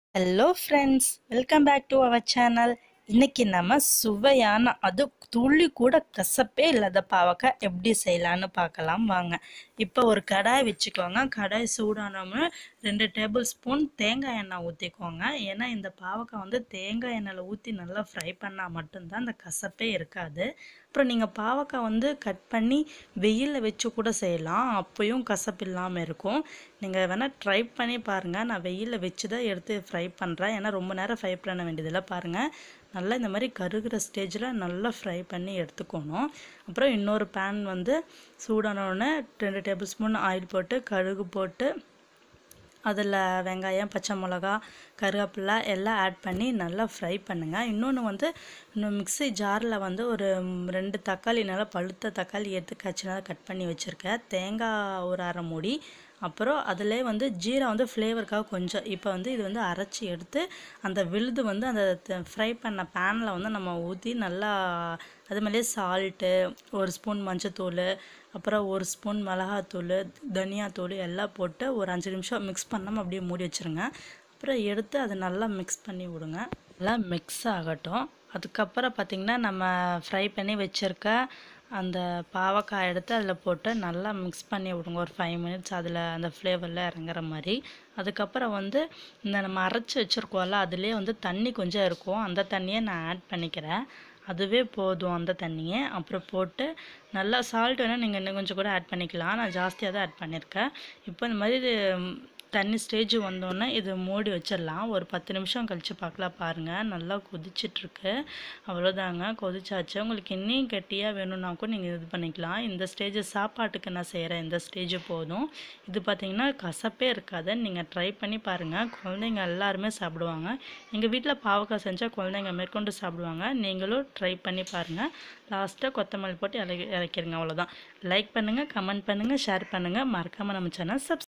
துளி கூட கசப்பே இல்லாத பாவக்காய் / bitter gourd recipes in Tamil../bitter gourd recipes....
Hi friends.
Try this bitter simple and tasty receipe in our bitter gourd.

Ingredients :

1. Bitter gourd - 2nos
2. Coconut oil - 2 table spoon
3. Mustard seed - 1 table spoon
4. Onion - 200g
5. Tomatoes - 2 nos
6. Coconut - 1 cup
7. Cumin seeds - 1 table spoon
8. Turmeric powder - 1 table spoon
9. Chilli powder - 2 table spoon
10. Some coriander leaves